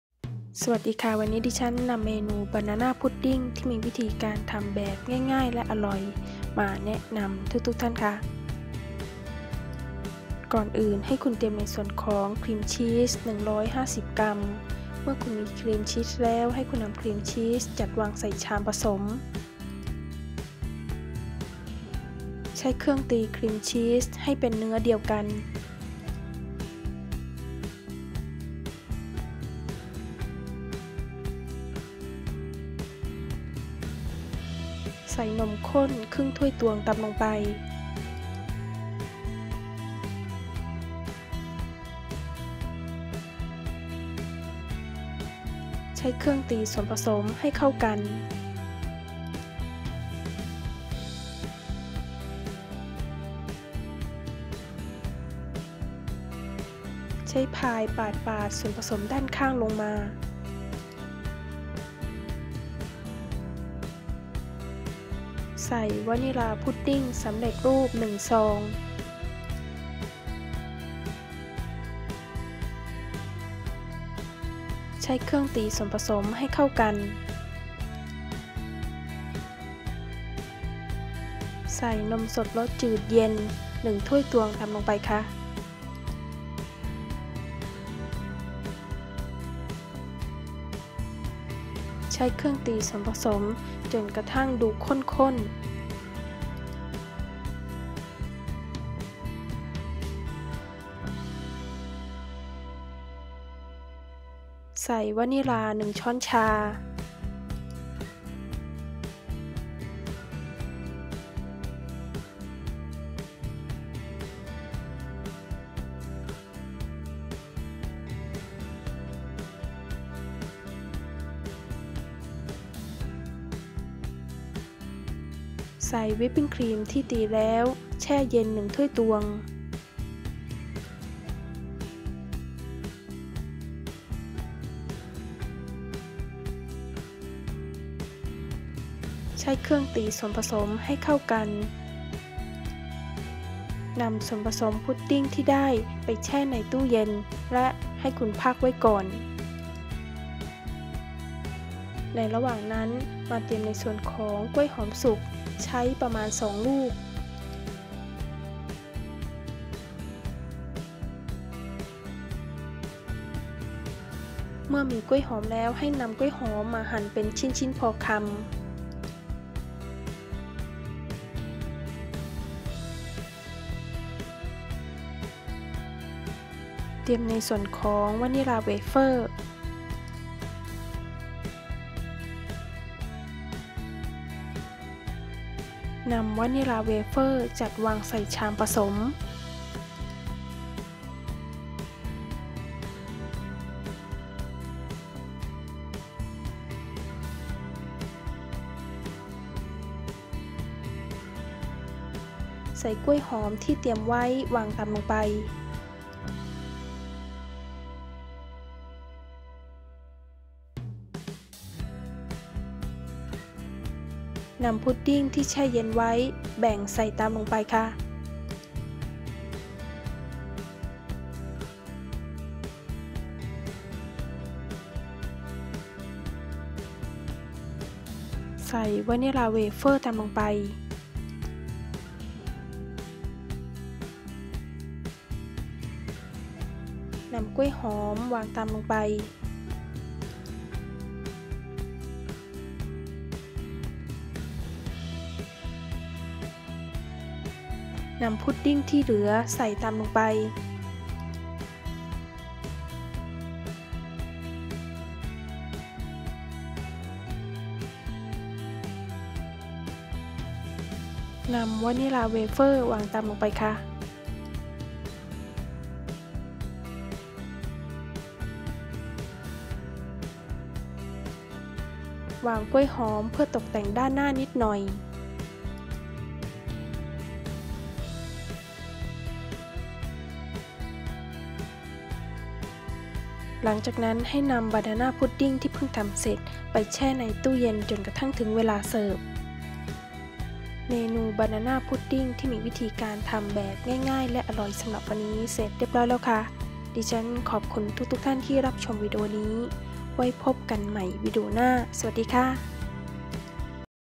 บานาน่าพุดดิ้ง (พุดดิ้งกล้วยหอม) : Banana Puddig Recipe
วีดีโอนี้สอนทำบานาน่าพุดดิ้ง (พุดดิ้งกล้วยหอม) : Banana Puddig Recipe